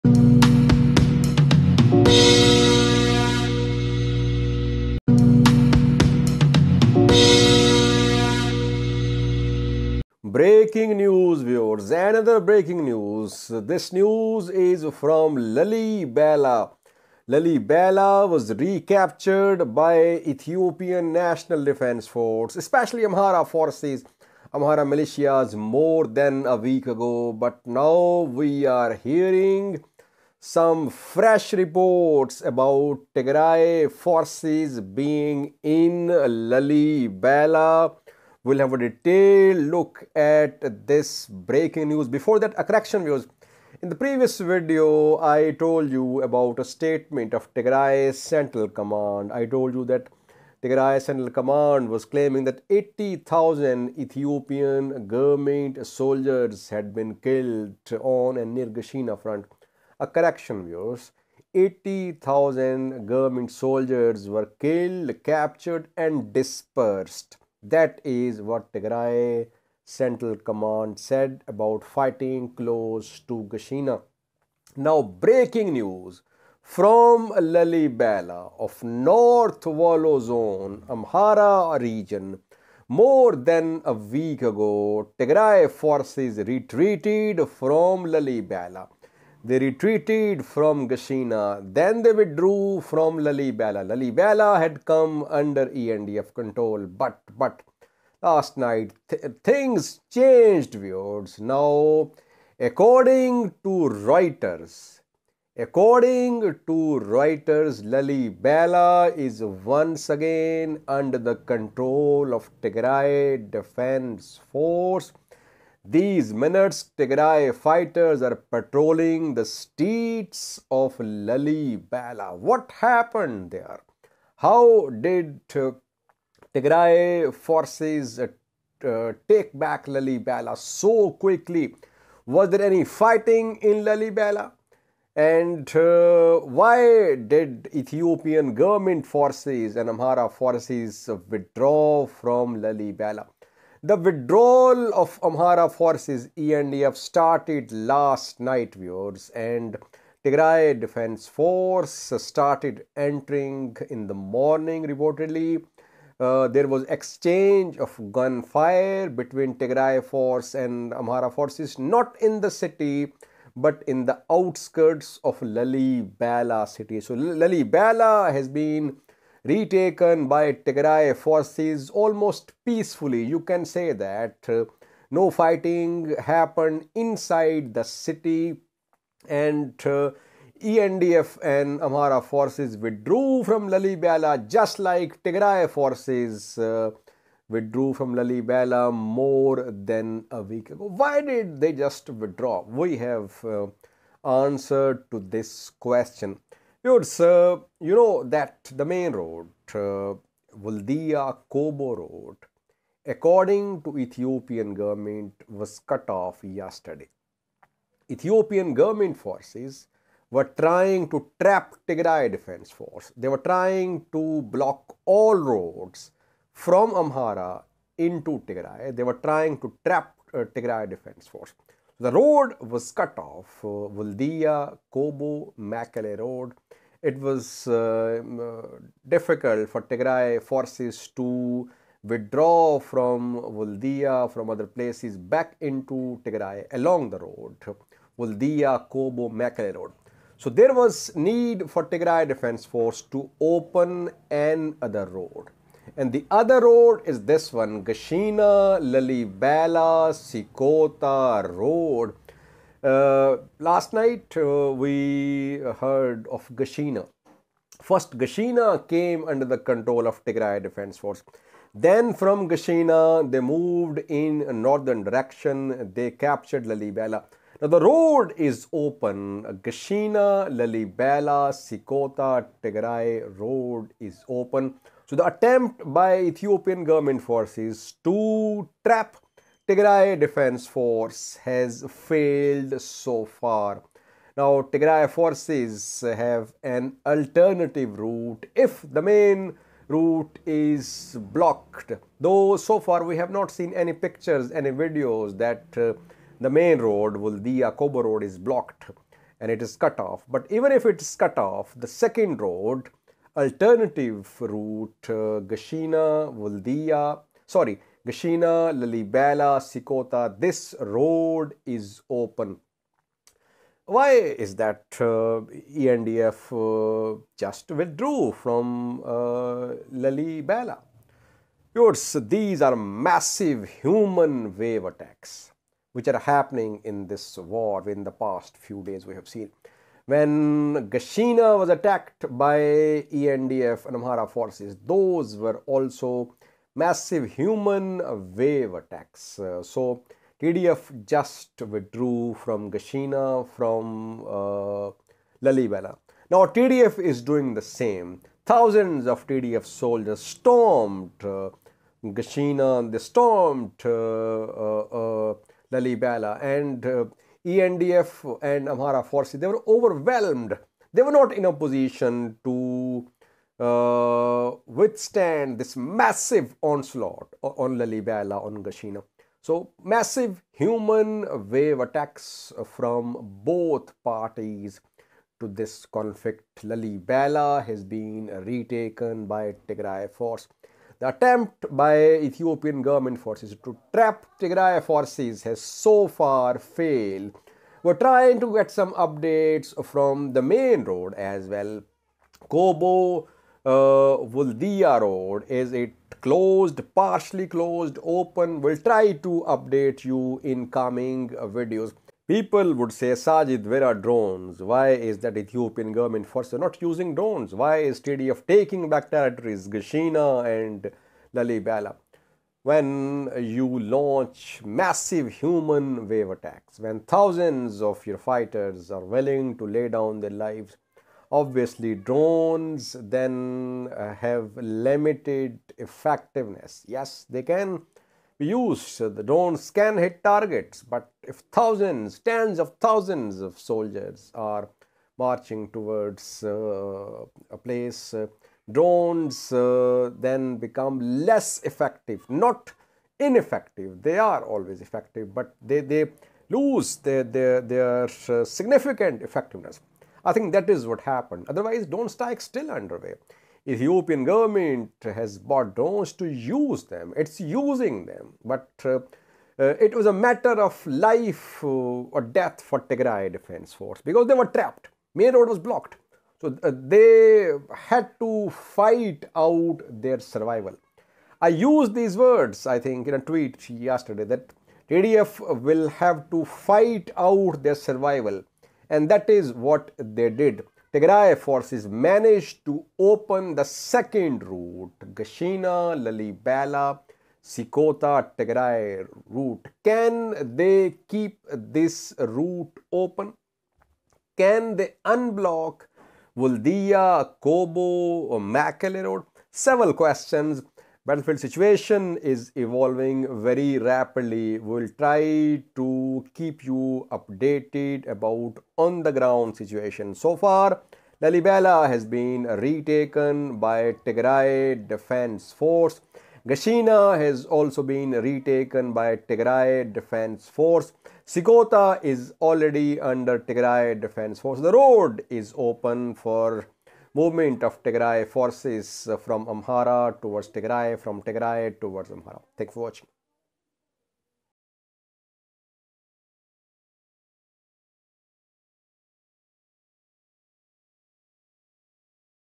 Ethiopia: Tigray forces retake Lalibela city of Amhara region
Tigray forces retake Lalibela city of Amhara region

For more of such videos and other updates about ongoing Tigray Ethiopia conflict, visit my